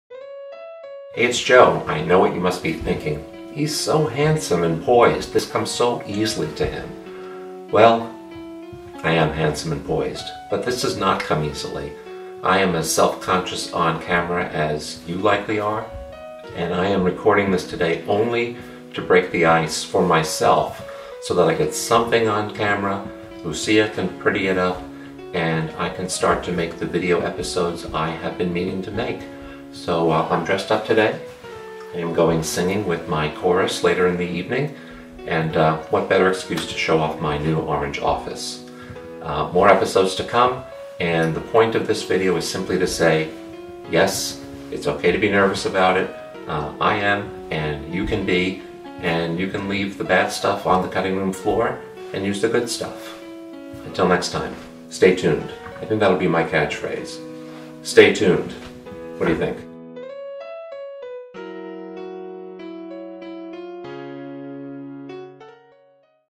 How to Start Making Videos for Your Business
At the risk of being too glib, the way to start is put on a camera and talk, dammit. There are hundreds of excuses why not. And heck, I'm not comfortable on camera either. But the power of video only increases with time. Do yourself and your medical device business a favor, go make a video. Even if it sucks. Just get started already.